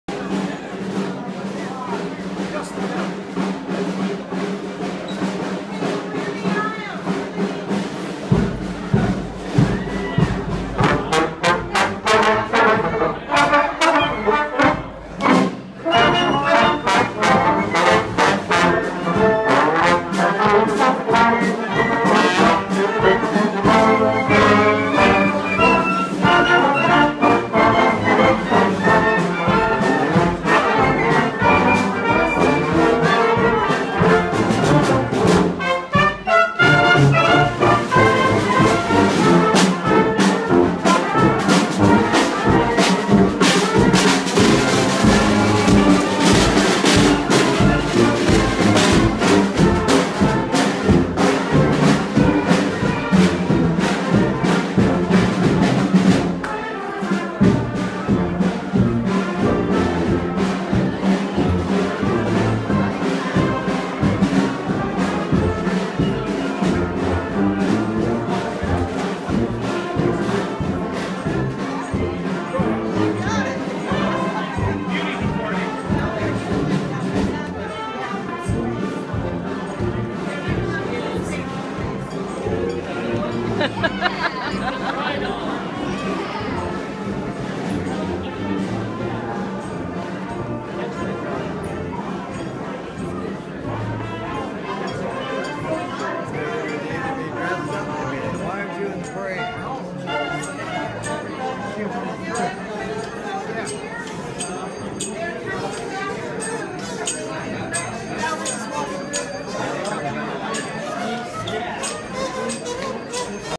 Grand March at the West Coast Ragtime Festival November 2015
Grand March at the at the West Coast Ragtime Festival, Saturday 21 November 2015, Rancho Cordova, California